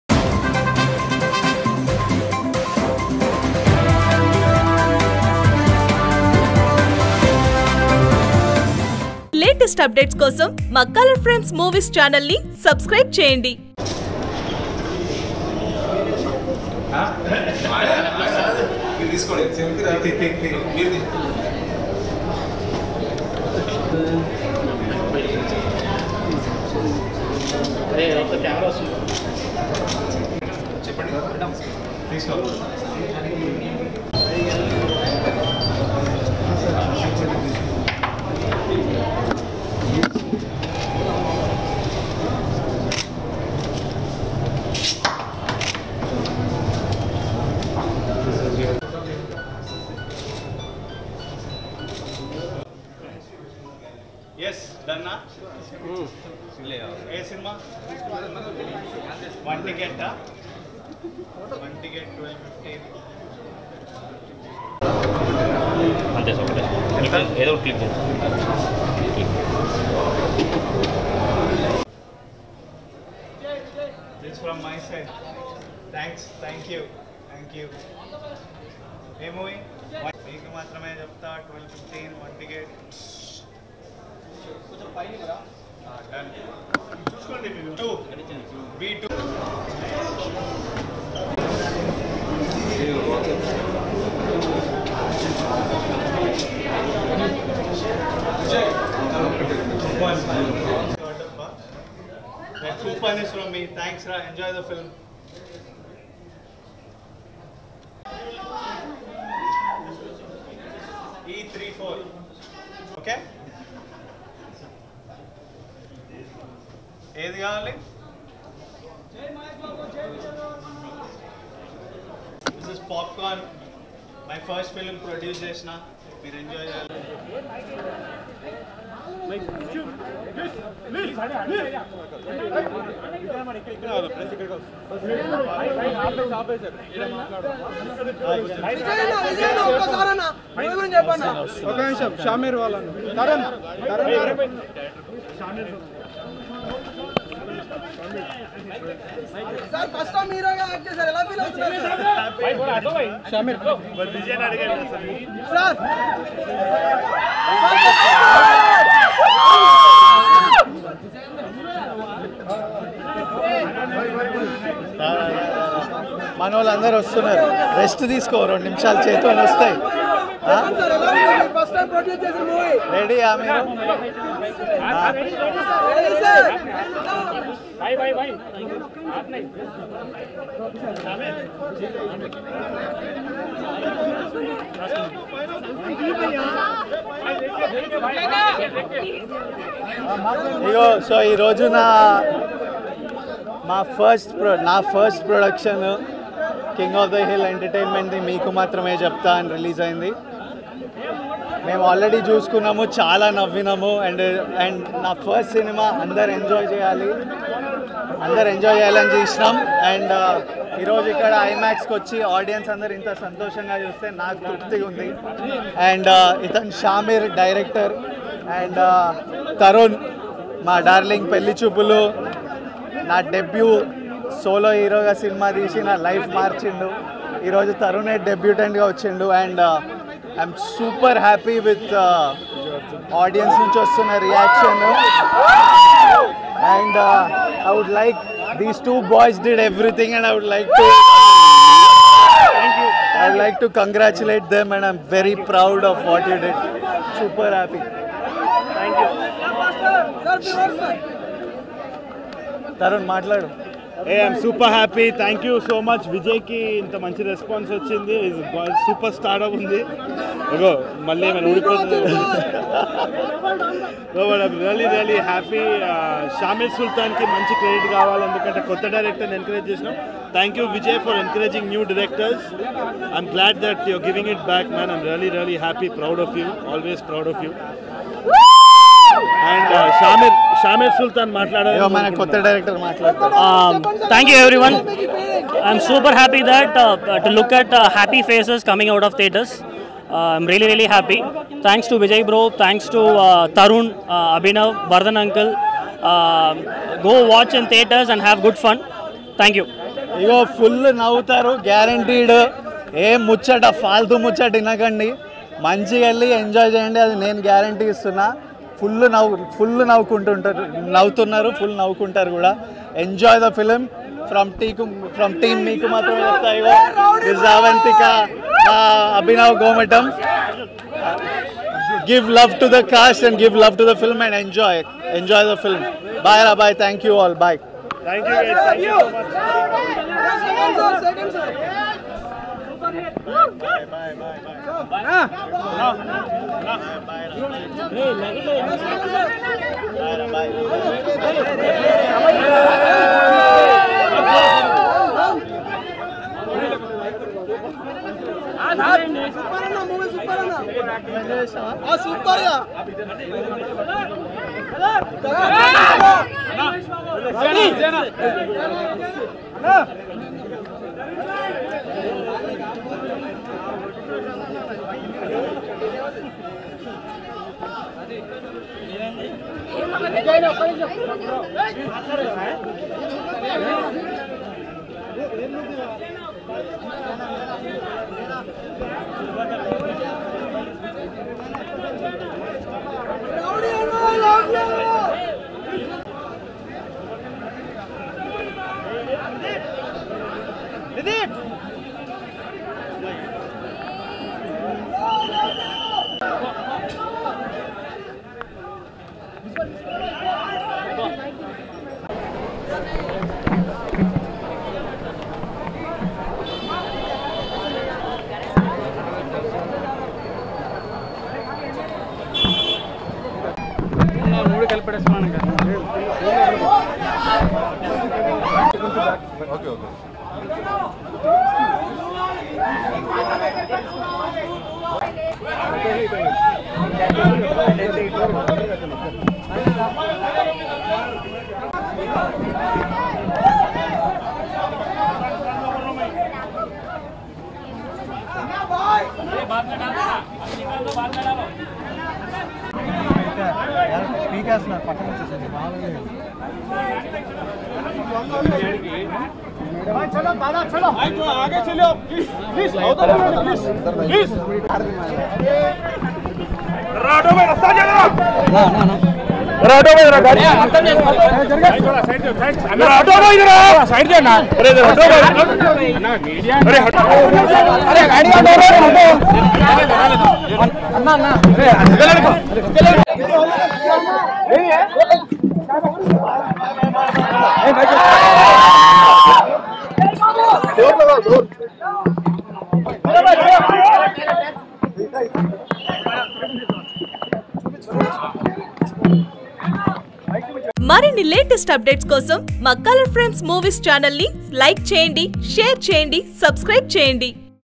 Vijay Devarakonda Hungama At IMAX | Meeku Maathrame Cheptha | విజయ్ దేవరకొండ హల్ చల్| CF Movies.
Watch Vijay Devarakonda hungama at imax theatre. Meeku Maathrame Cheptha.